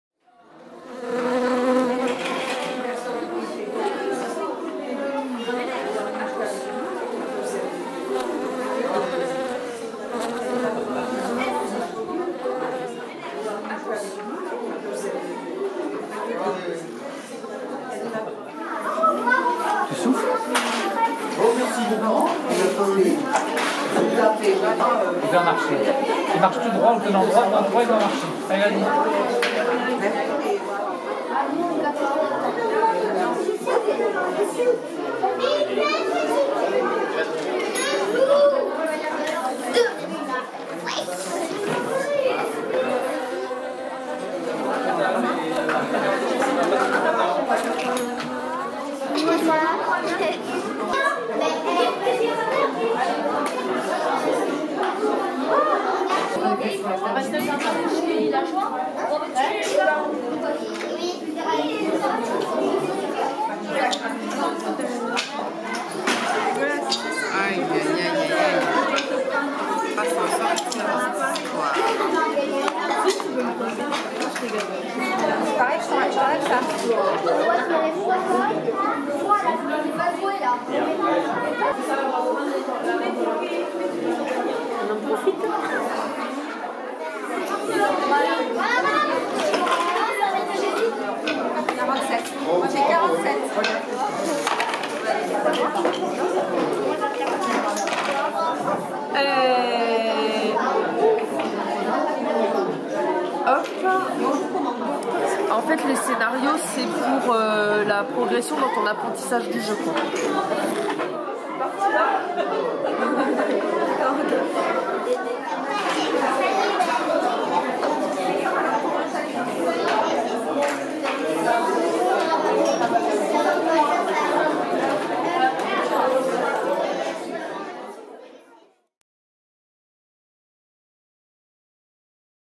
123JOUONS
2018-Reportage rapide sur une présentation de jeux aux enfants et adultes.
Tous les participants ce sont bien amusés.
Association organisatrice : Pouss'les Murs